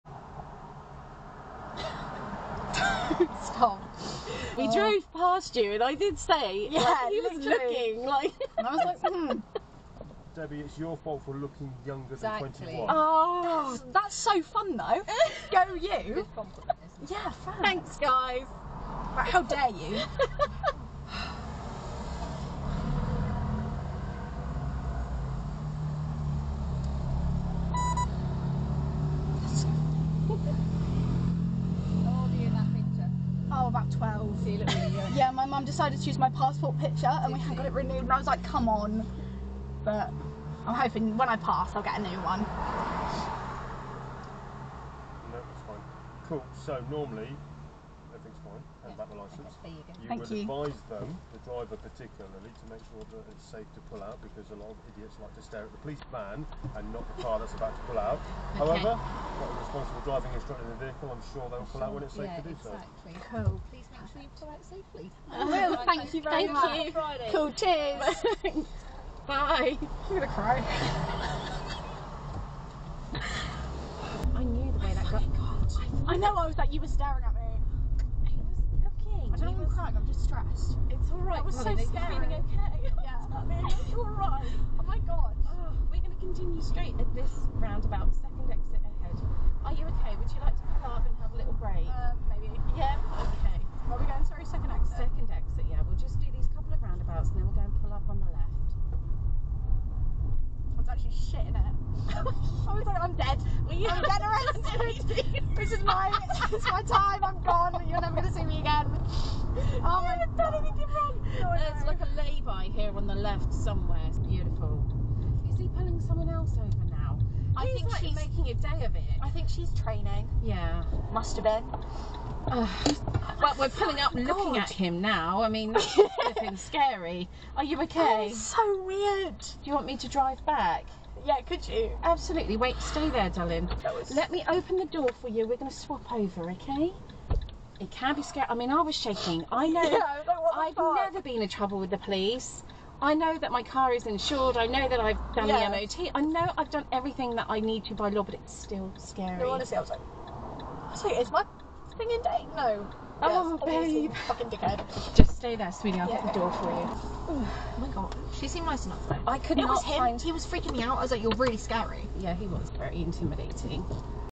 Part 3 our experience of being pulled up by the police!
This was quite upsetting for me, seeing Hannah so upset for something which was obviously a training exercise. I do however wish the female officer all the best for the future, she was very polite and relatable.